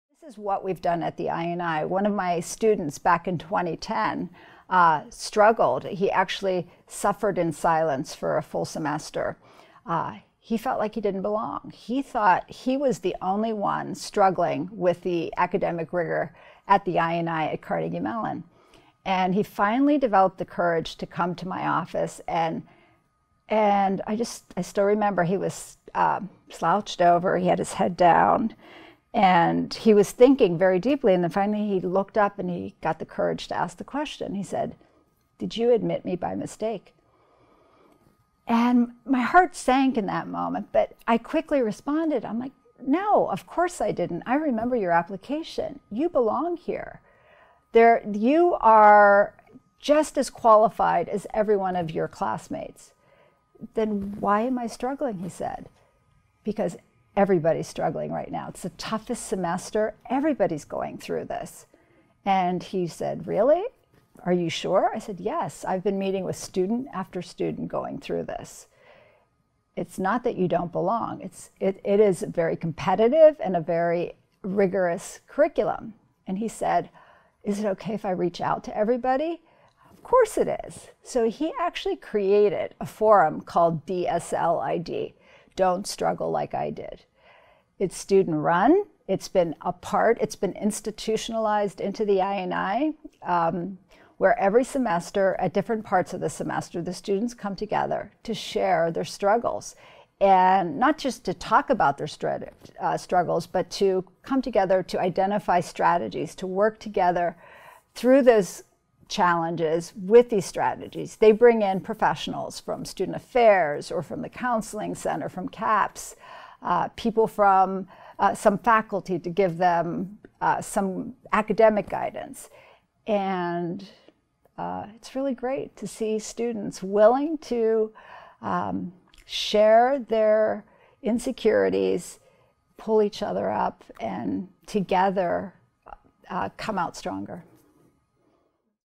The Student Who Inspired Me
As director of the Information Networking Institute at Carnegie Mellon University, Dr. Dena Haritos Tsamitis has observed impostor syndrome with many of her engineering graduate students, but it’s the story of one in particular that inspired her the most and compelled her to look within and examine my own feelings of self-doubt.

Watch as she charts the journey of the student who created an INI student group called "Don't Struggle Like I Did" and then read her LinkedIn blog for more on impostor syndrome